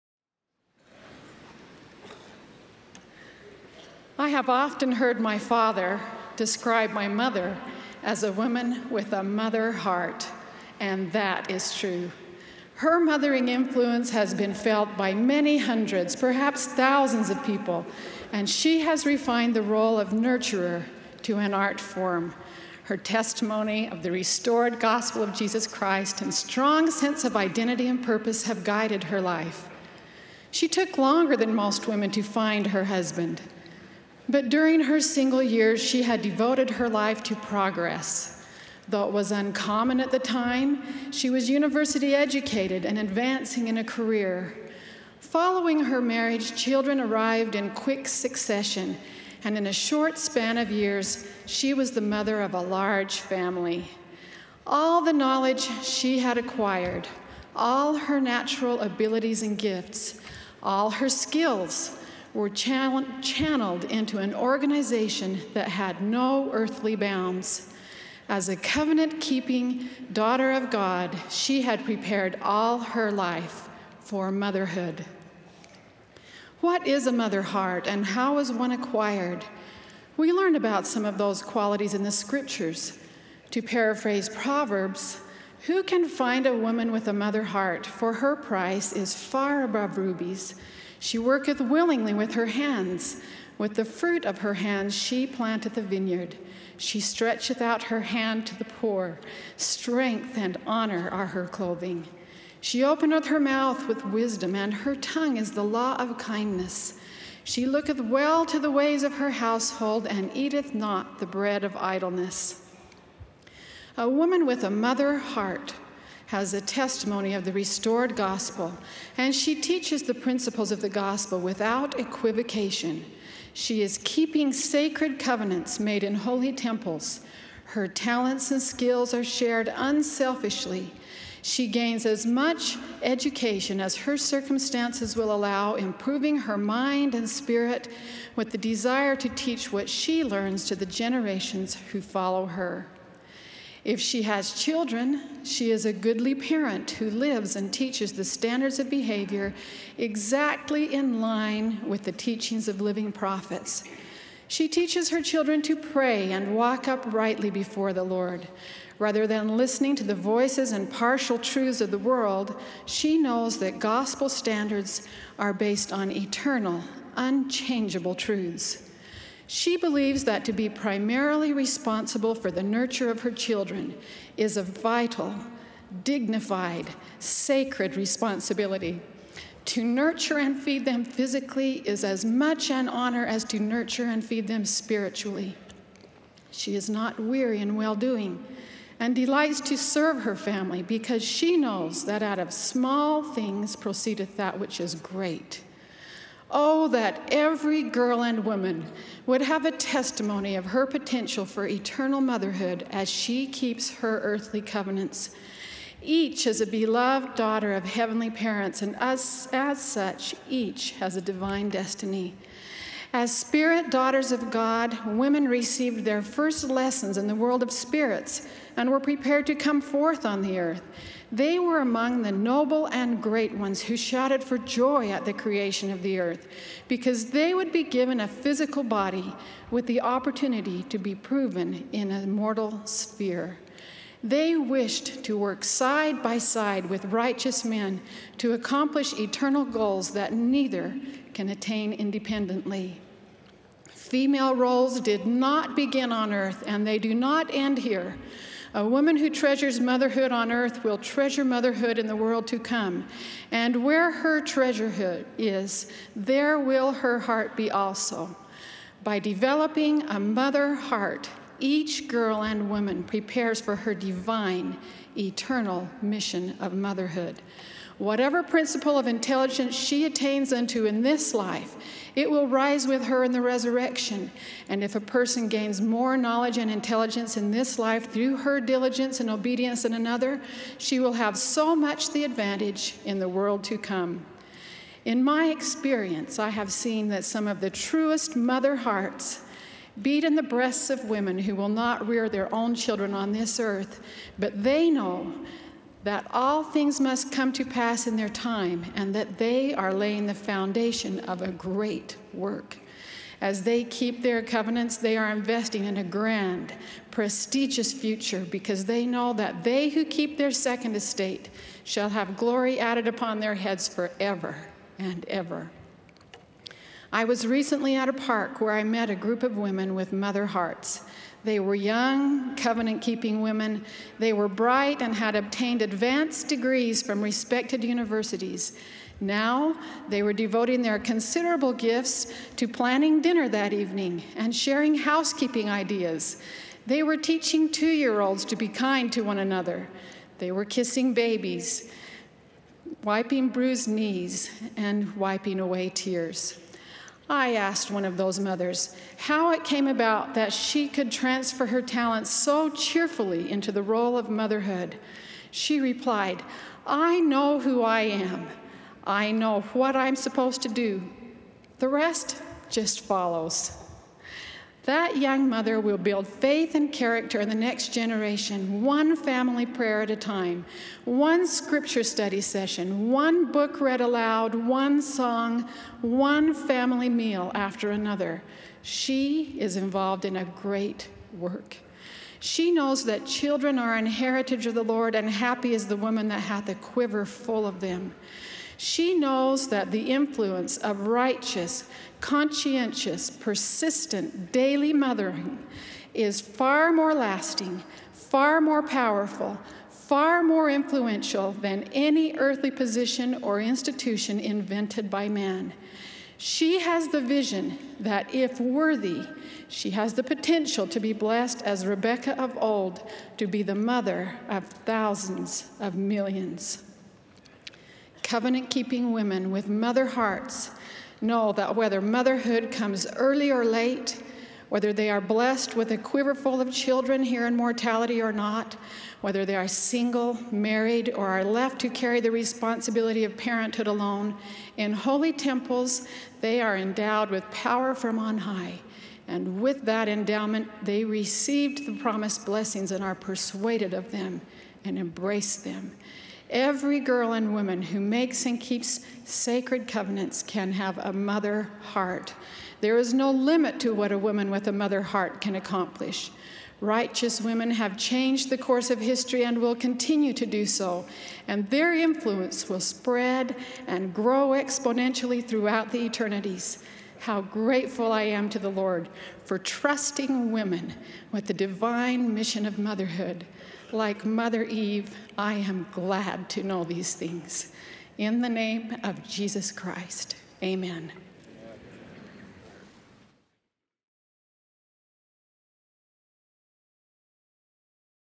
A “Mother Heart
Julie B. Beck - By developing a mother heart, each girl and woman prepares for her divine, eternal mission of motherhood.